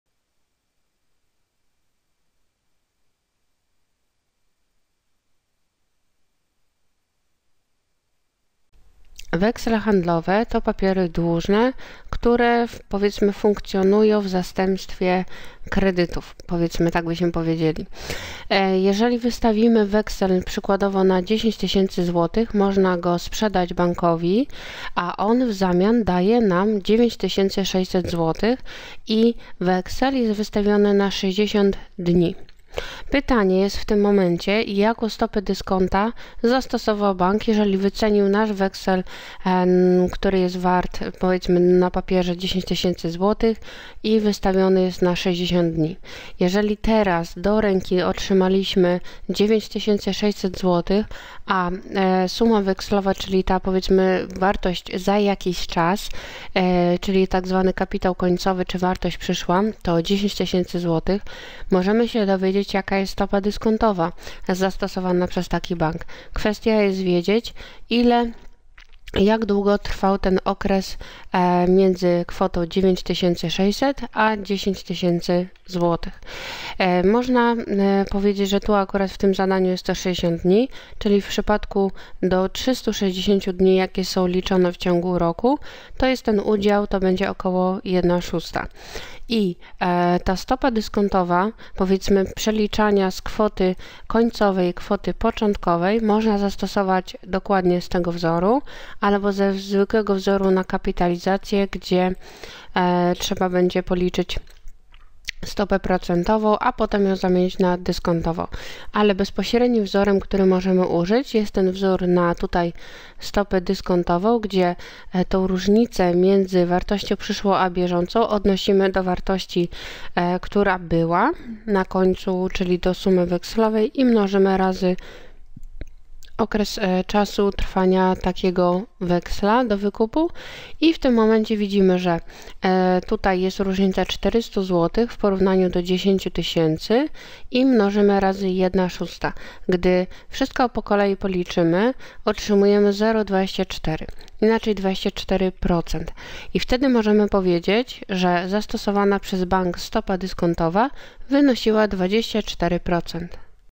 Stopa dyskontowa weksla handlowego przy wykupie przez bank
Na przykładzie pokazuję, jak sprawnie można oszacować stopę dyskontową w przypadku przedstawienia weksla handlowego do wykupu.